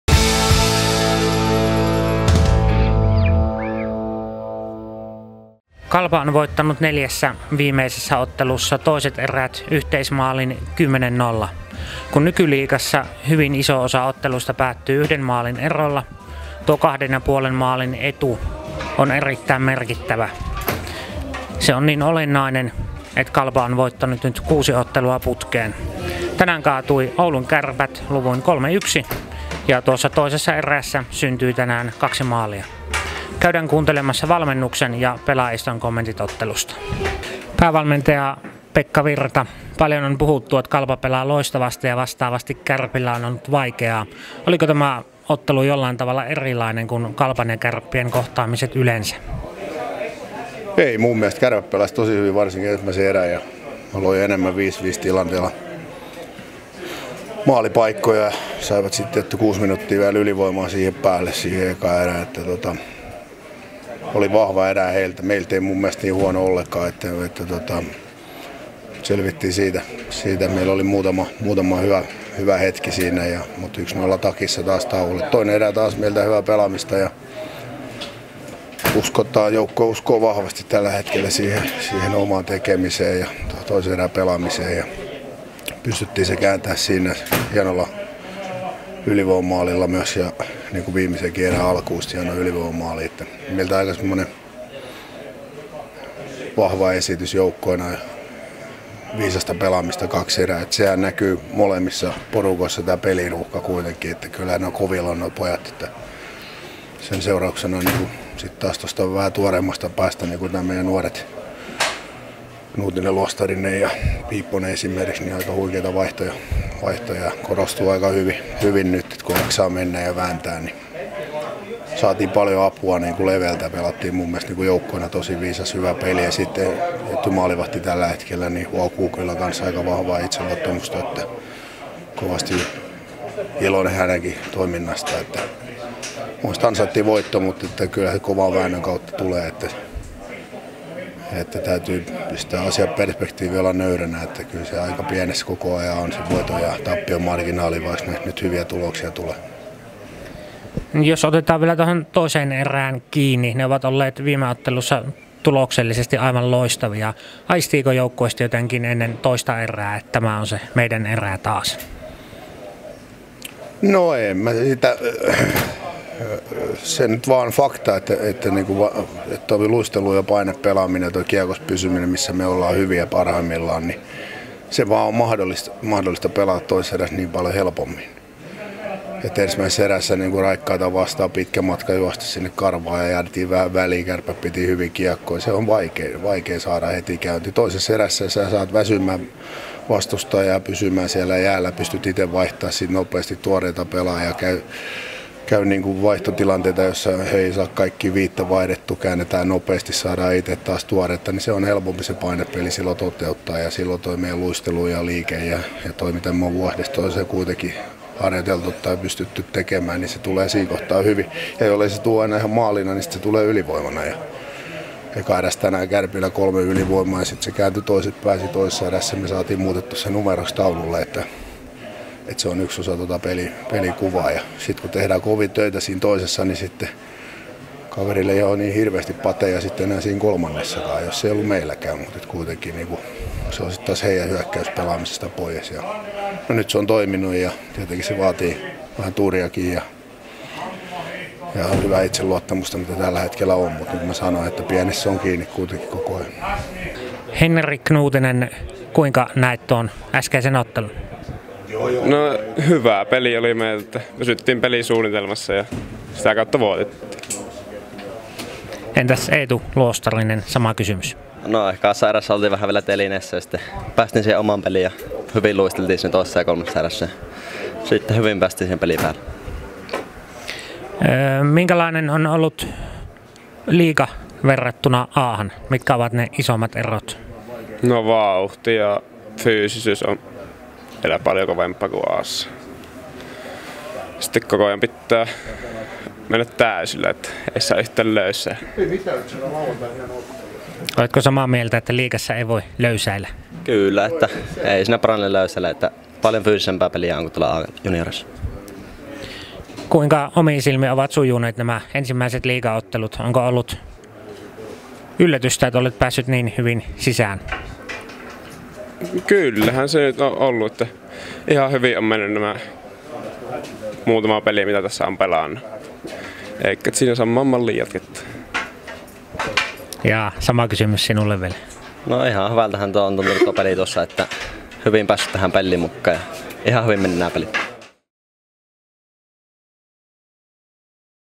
Hikihaastattelu: Päävalmentaja Pekka Virta ja hyökkääjät Eetu Luostarinen ja Henri Knuutinen
KalPa jatkoi voittoputkeaan jo kuuden ottelun mittaiseksi, kun Kärpät kaatui kotona luvuin 3-1.